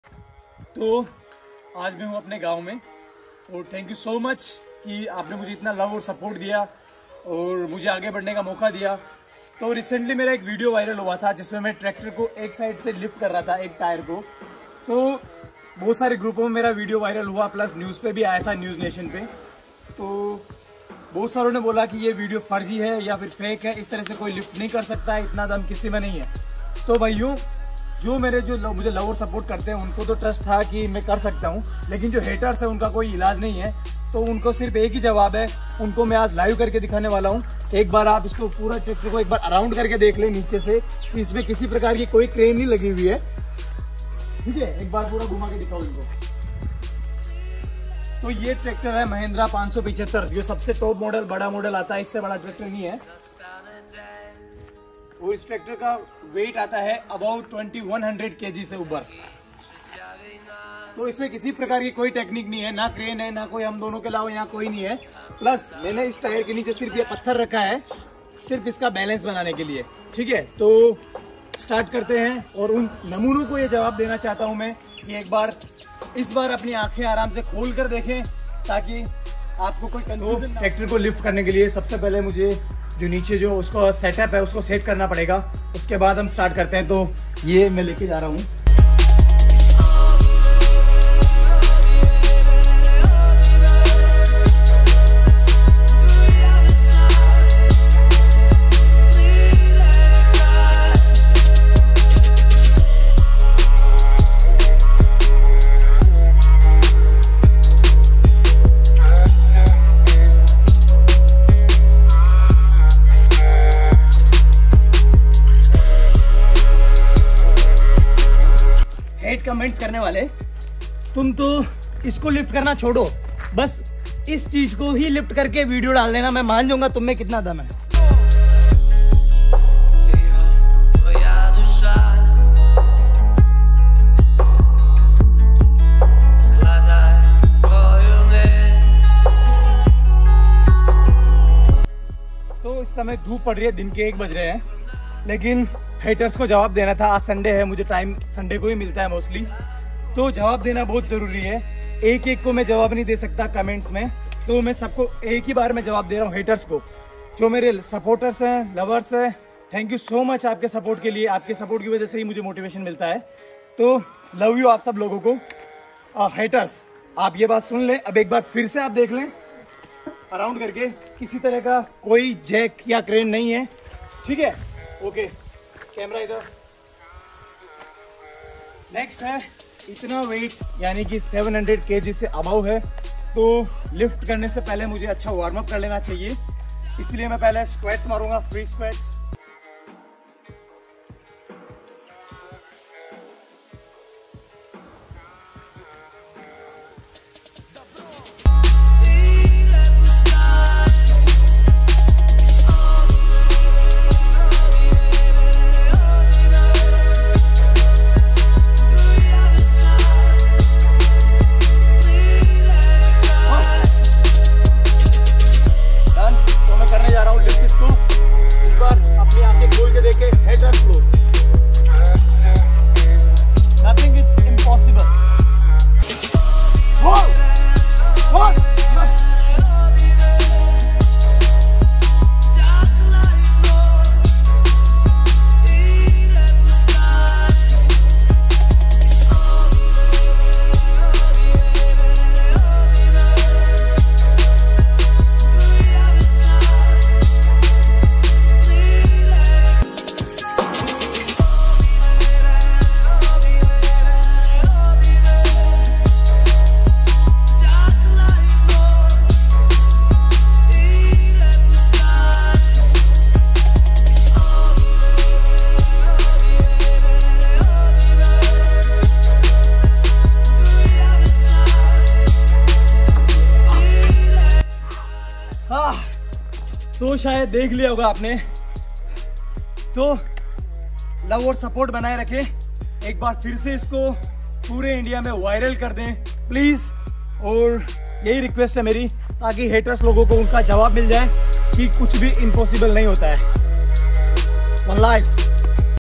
Pawan Sahu viral video || Pawan Sahu gym ||. Pawan sahu motivation || stone fitness gym Bhilwara
To m 1 year ke bad vapas you tube video upload kr raha hu or mera ek hi target ha is video ka Ki Kuch bhi impossible nhi hota ha agr hm Kuch krna chahe to or durse bat mere haters ko ye Javab ha Ki ye koi fake nhi ha  thank you so much mere Bhaiyo ko jo muje love or support krte ha or is video ko support de taki m apko or motivate kr SKU